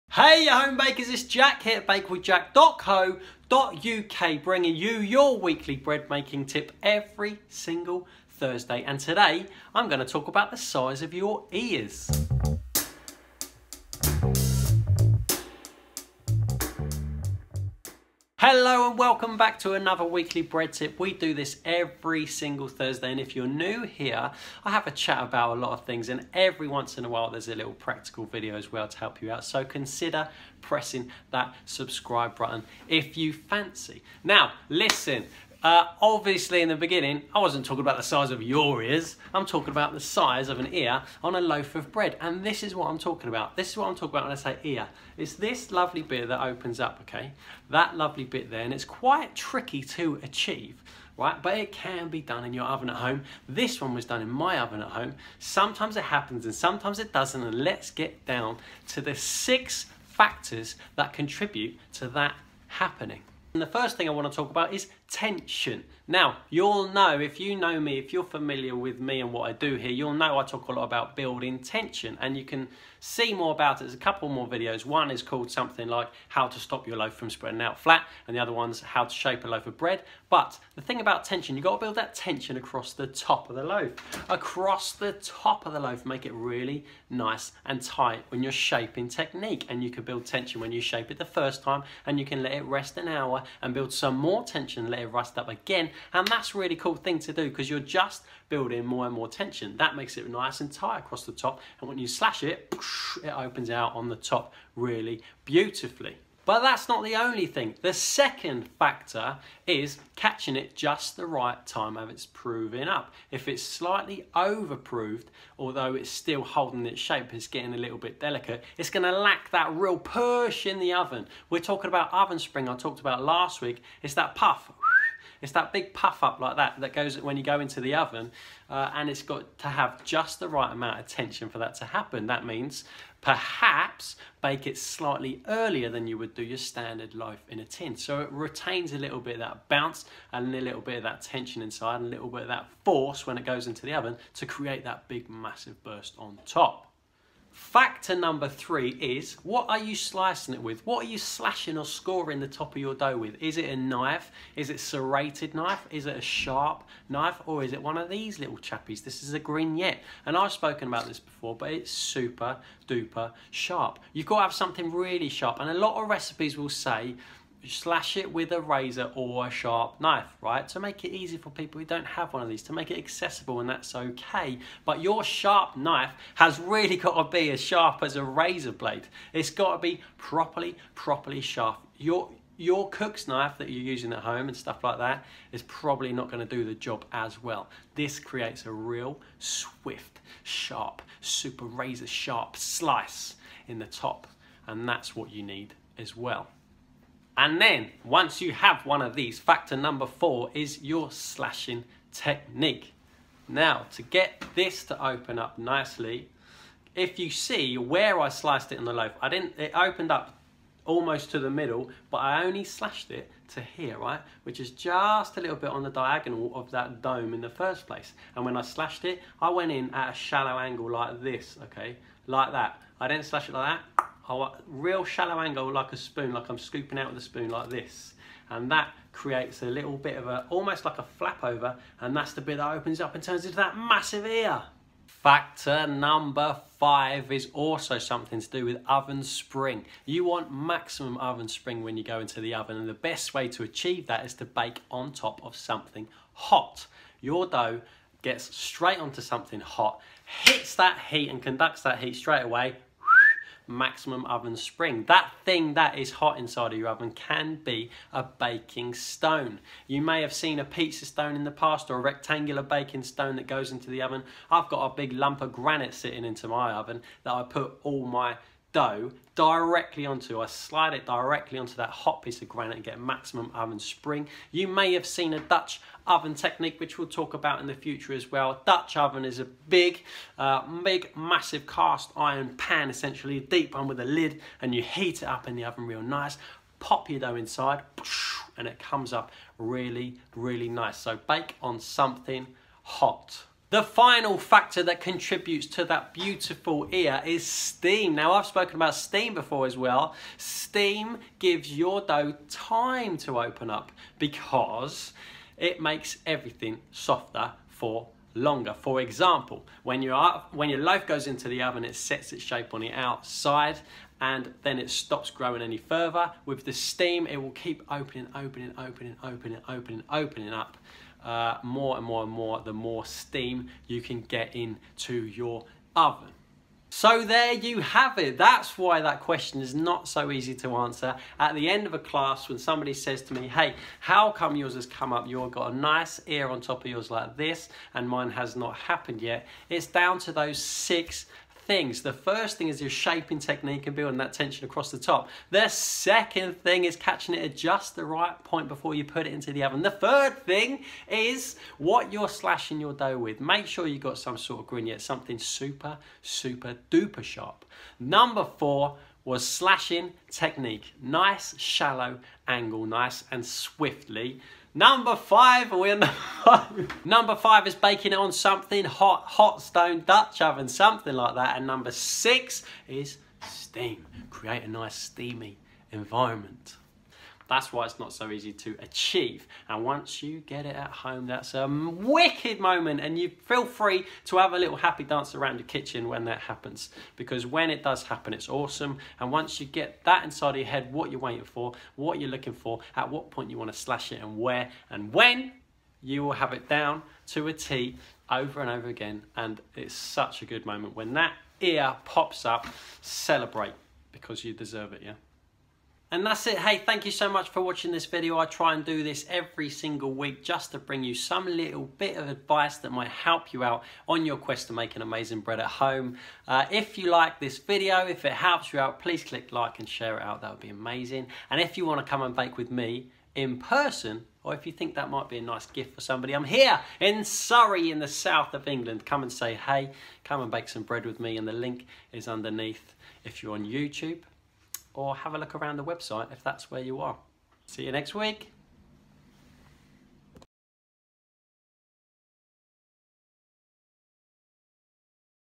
42: How to get a good EAR on your loaf - The SIX main factors - Bake with Jack
Amazon US
Amazon CA